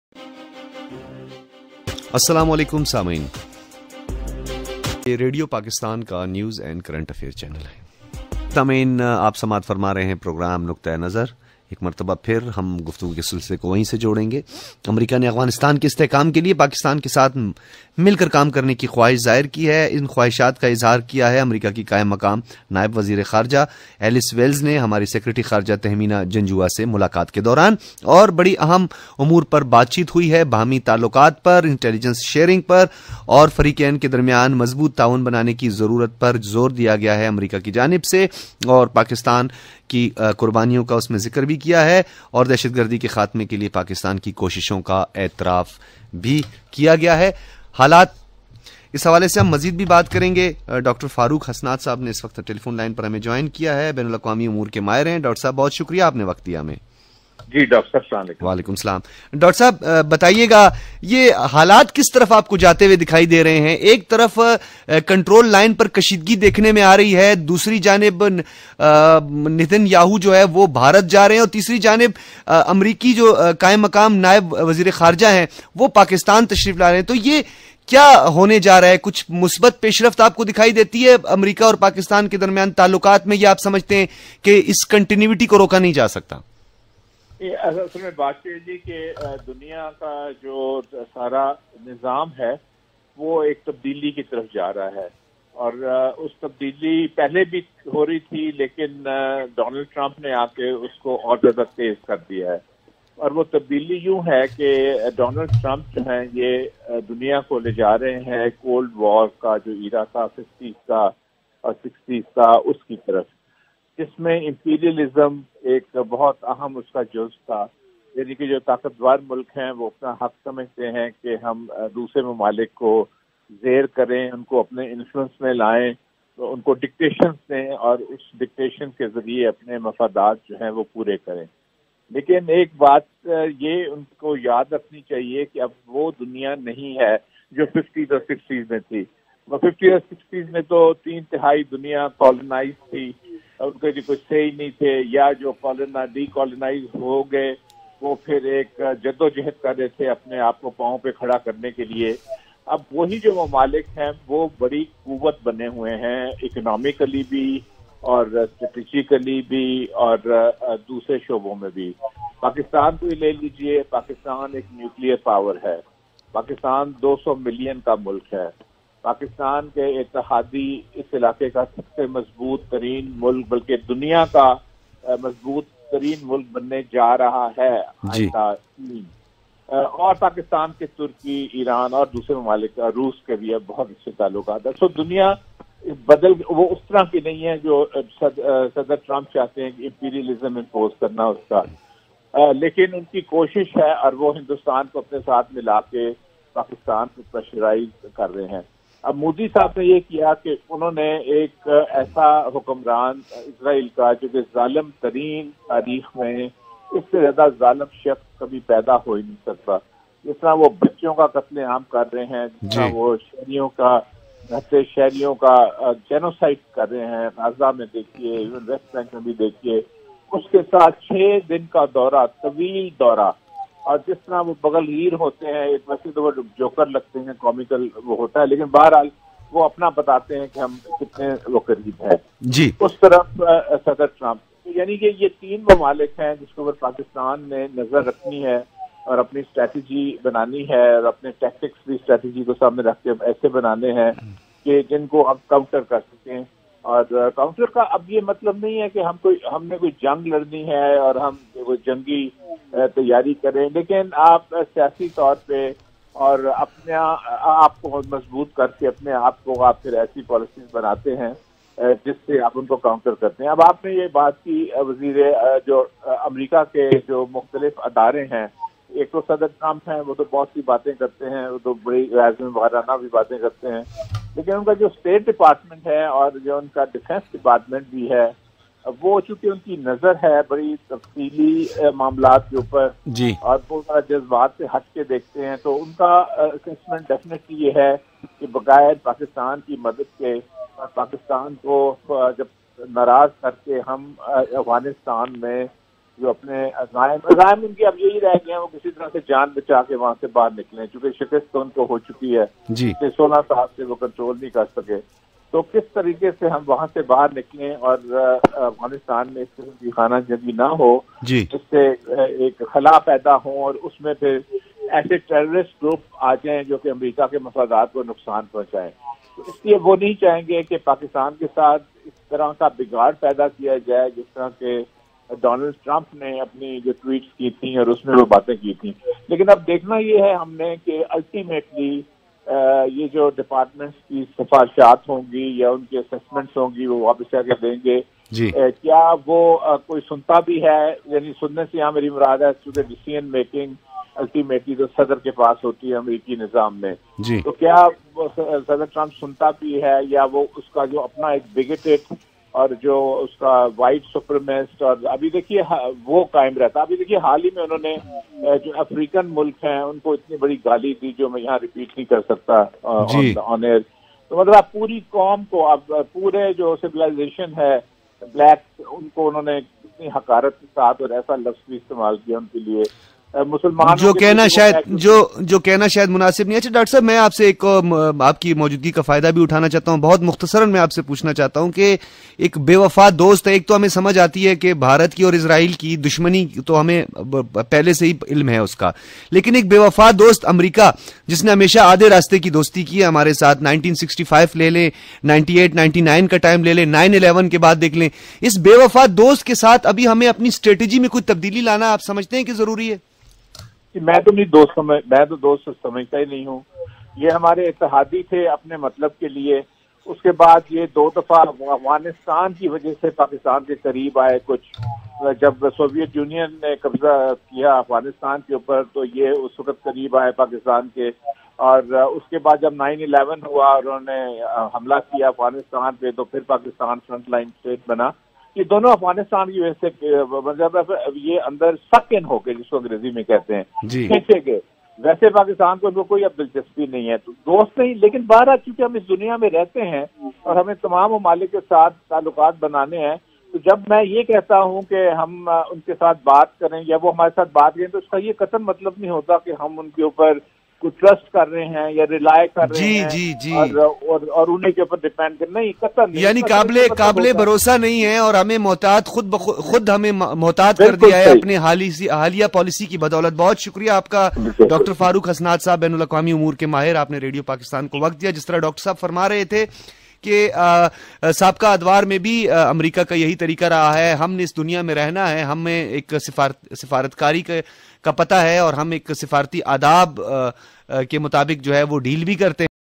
PAKISTAN - US COMPLICATIONS -DR FAROOQ HASNAT- JAN 15,2018-RADIO PAKISTAN
PAKISTAN-US COMPLICATIONS
پاکستان اور امریکه  کے درمیان الجھائو
DR FAROOQ HASNAT       ڈاکٹر فار ق حسنات
RADIO PAKISTAN – JANUARY 15, 2018